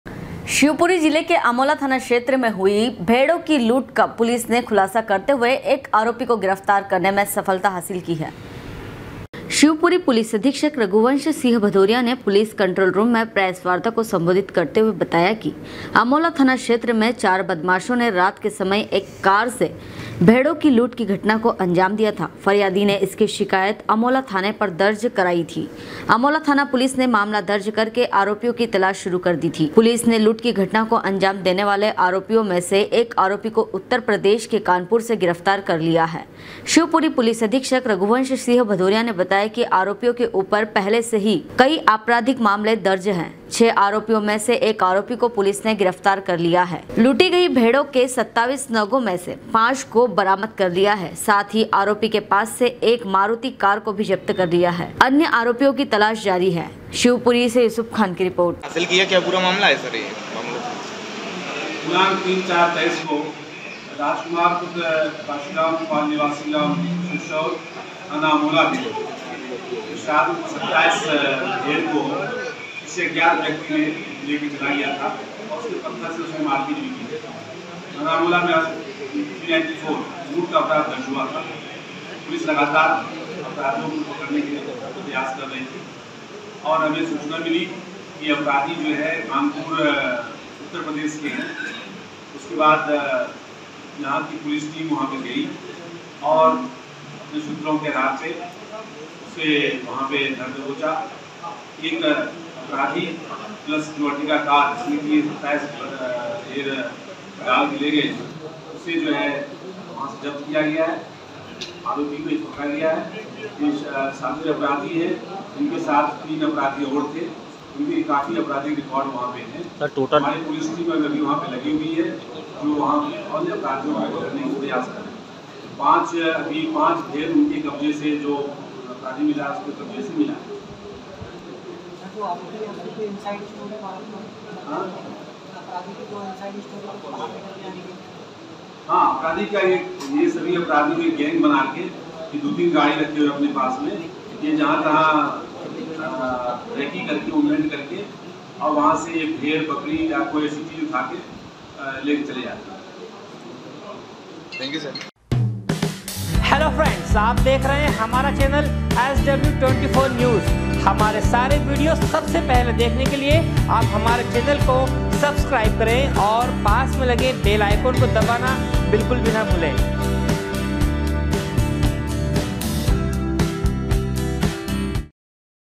शिवपुरी के अमोला थाना क्षेत्र में हुई भेड़ो की लूट का पुलिस ने खुलासा करते हुए एक आरोपी को गिरफ्तार
शिवपुरी जिले के अमोला थाना क्षेत्र में हुई भेड़ो की लूट का पुलिस ने खुलासा करते हुए एक आरोपी को गिरफ्तार करने में सफलता हासिल की है।
शिवपुरी से युसूफ खान की रिपोर्ट।